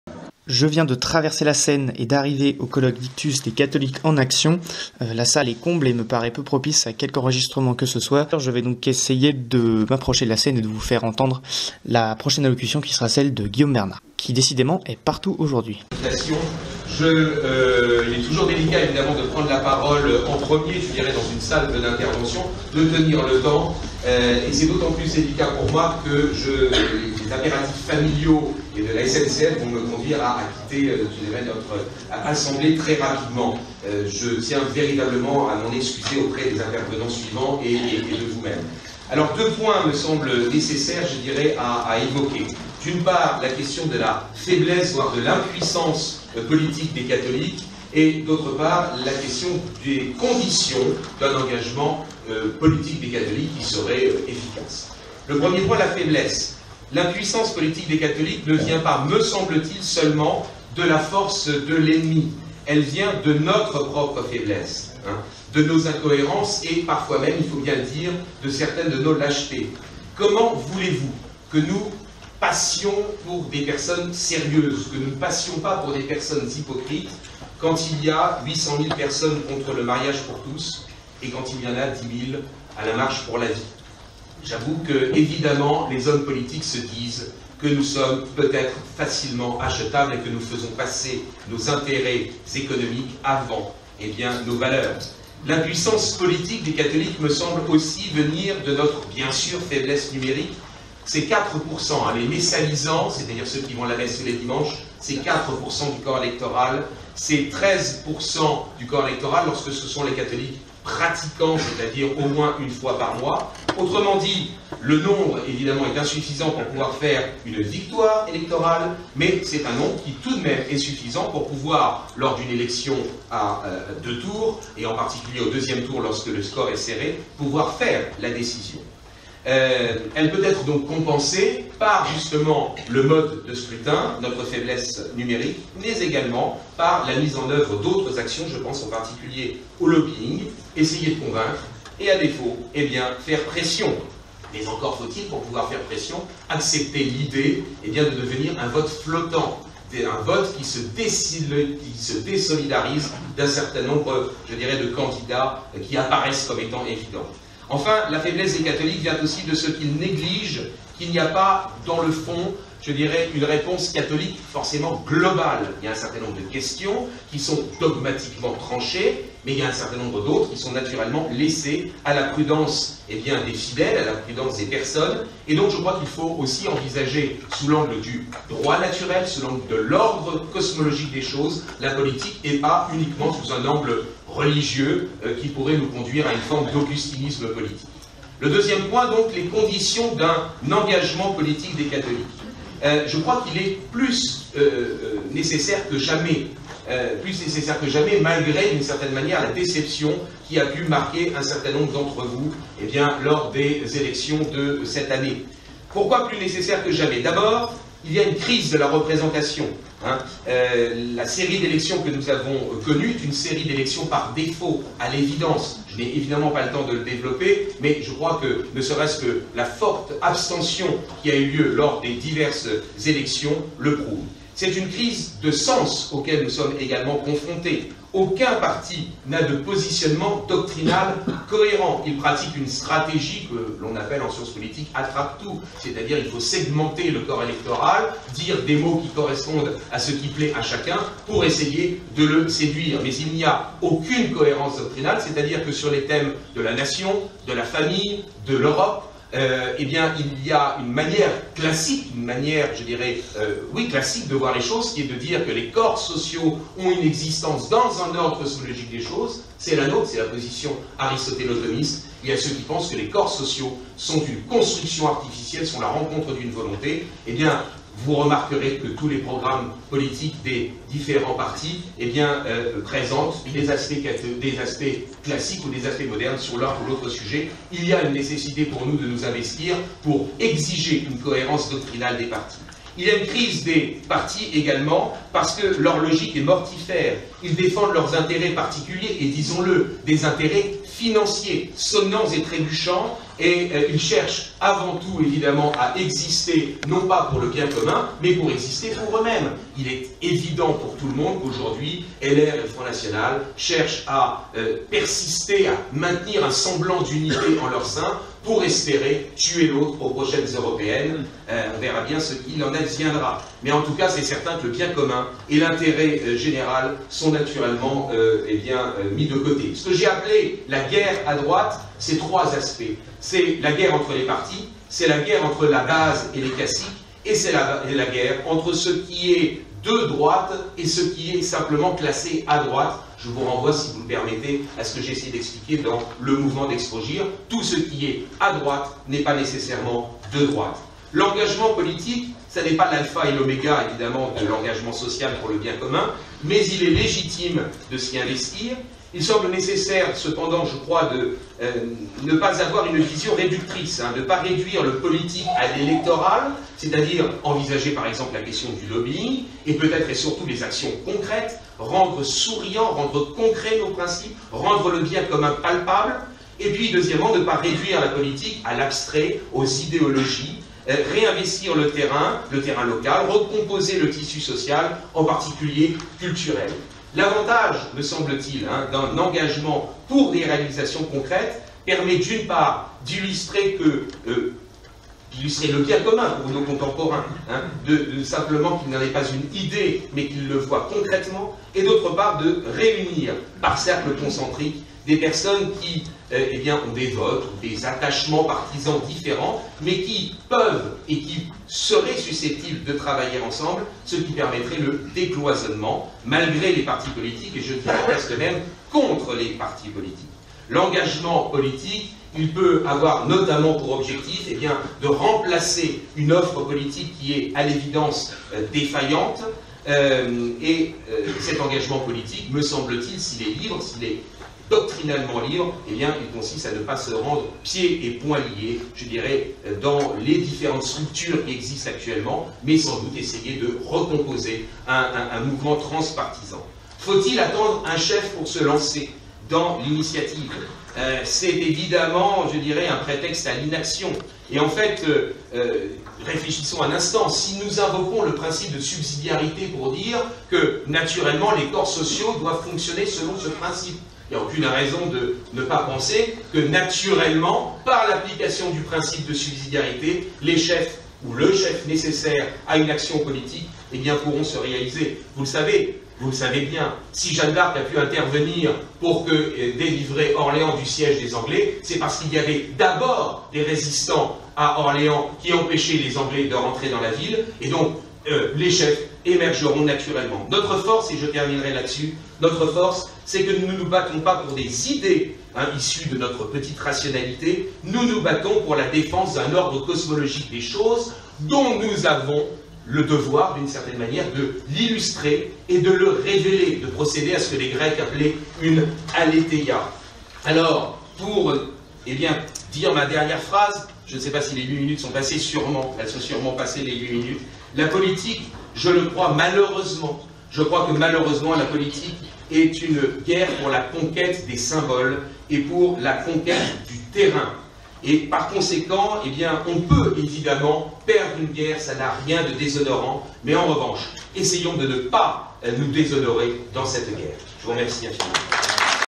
Guillaume Bernard sur l'engagement politique des catholiques (novembre 2017)
Intervention de Guillaume Bernard au colloque d'Ichtus, à Paris, le 18 novembre 2017.